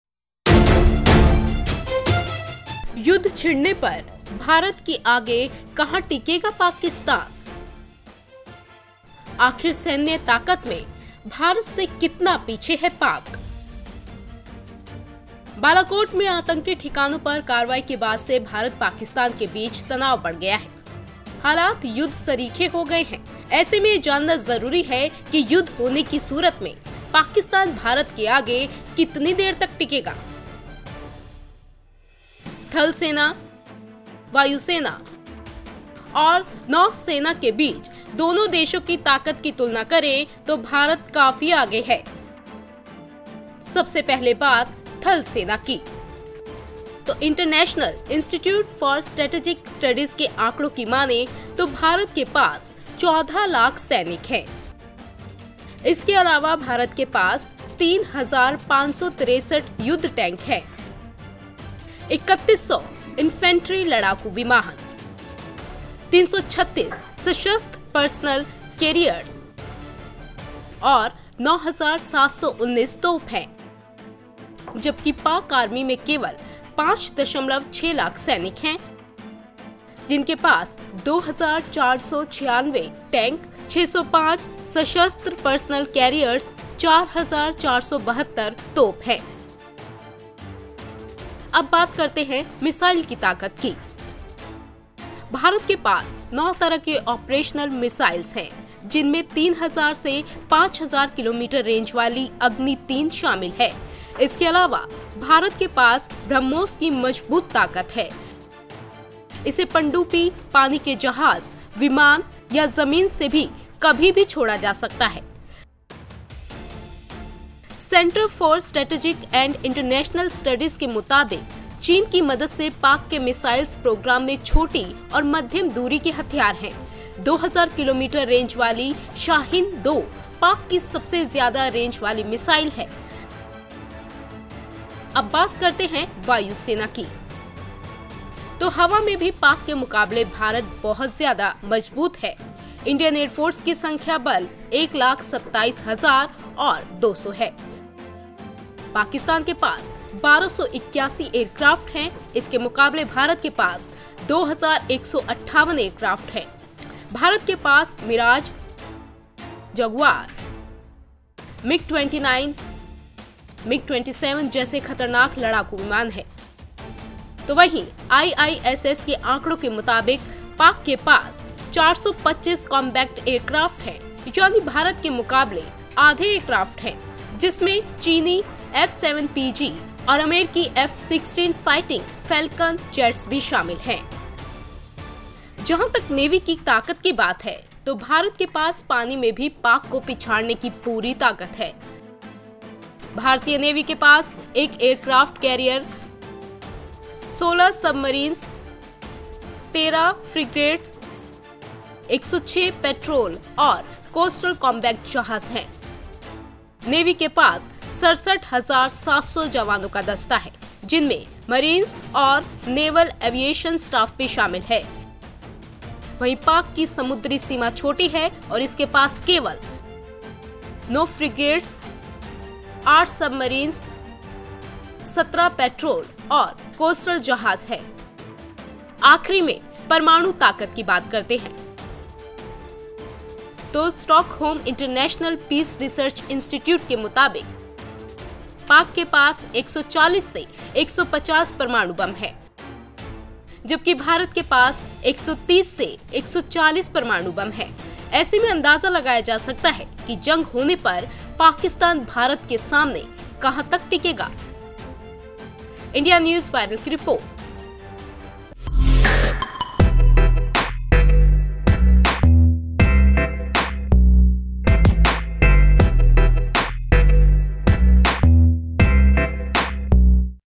युद्ध छिड़ने पर भारत के आगे कहां टिकेगा पाकिस्तान ? INDIA NEWS VIRAL
युद्ध छिड़ने पर भारत के आगे कहां टिकेगा पाकिस्तान ? आखिर सैन्य ताकत में भारत से कितना पीछे है पाकिस्तान ? देखिए इस रिपोर्ट